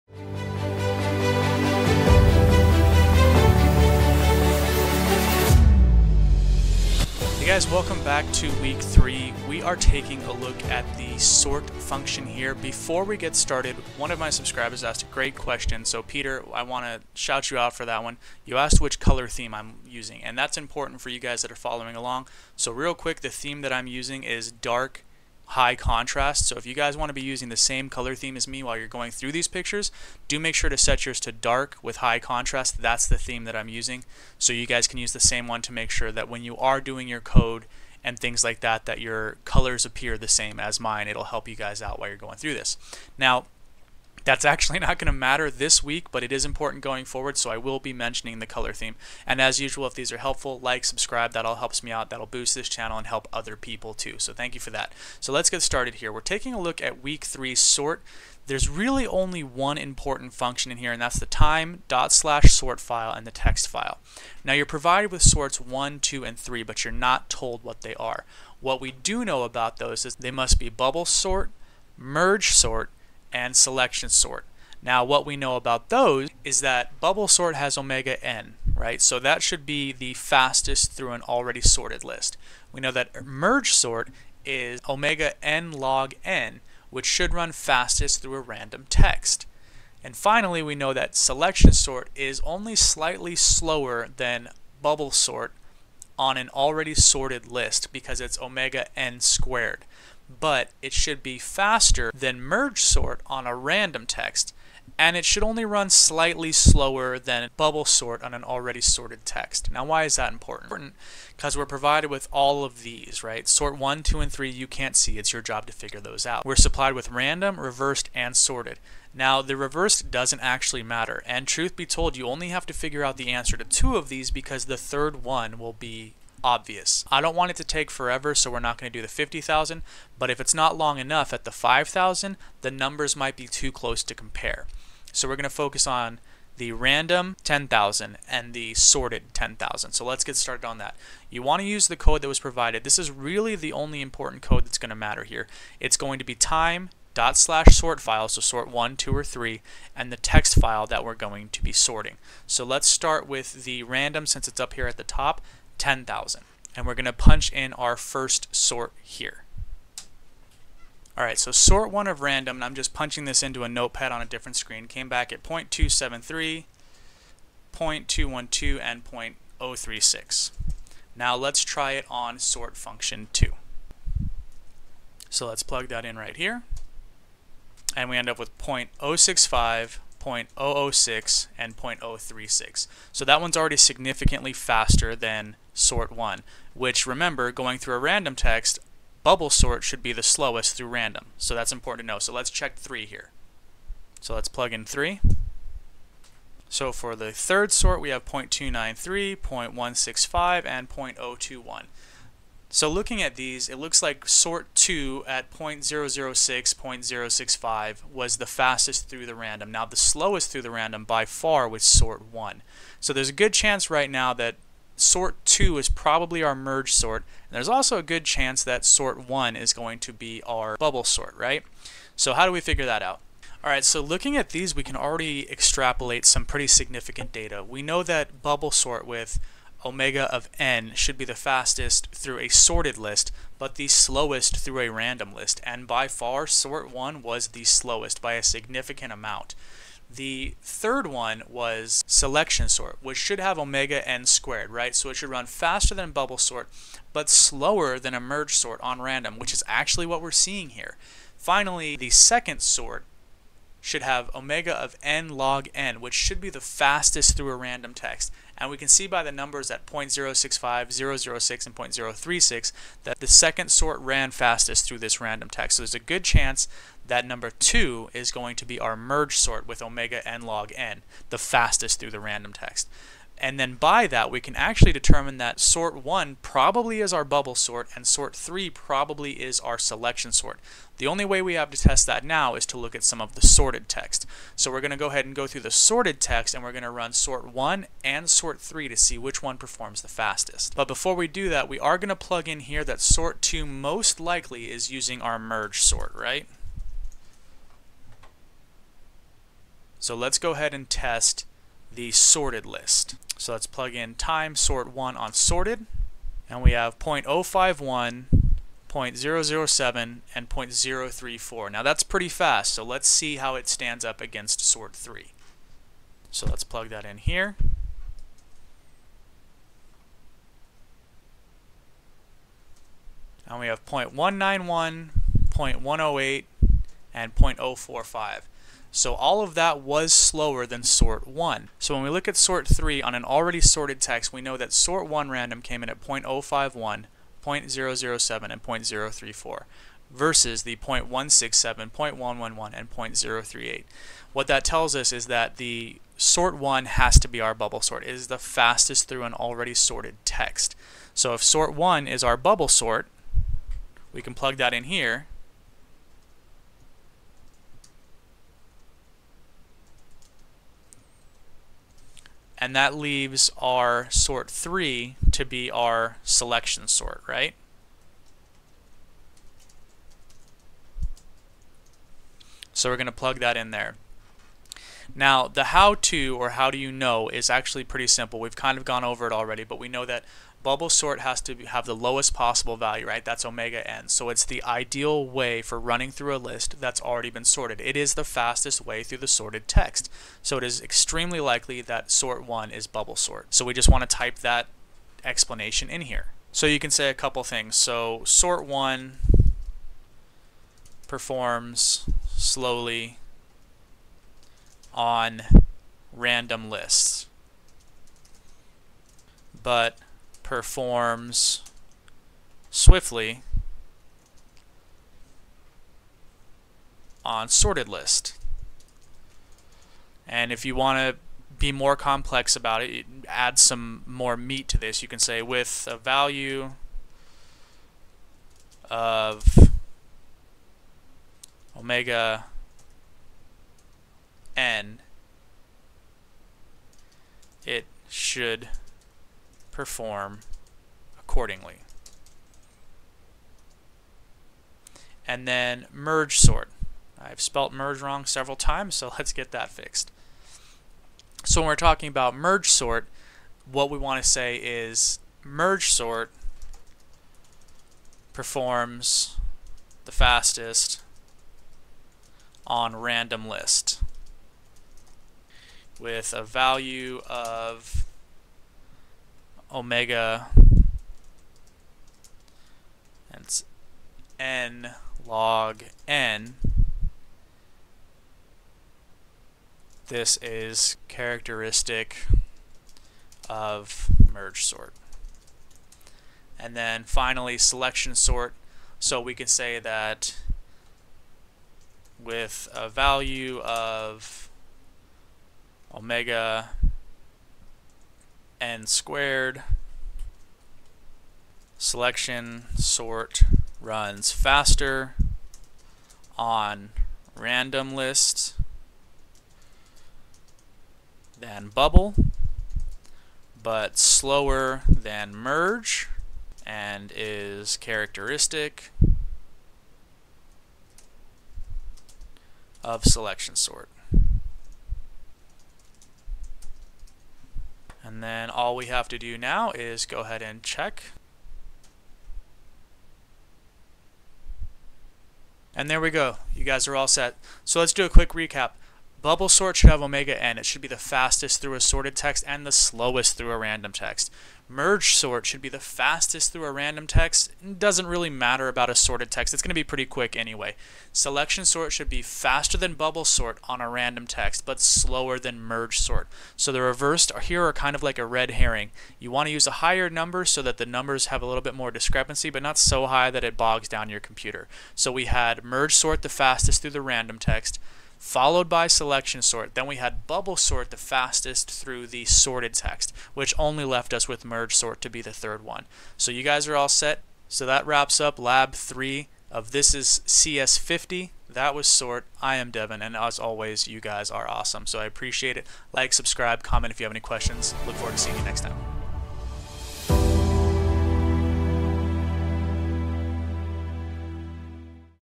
CS50 Sort 2024 - Week 3 Lab - Sort Solution 2024. (Beginners Guide)
Welcome to This is CS50 Week 3 Lab - Sort. This tutorial will cover how to complete CS50x Sort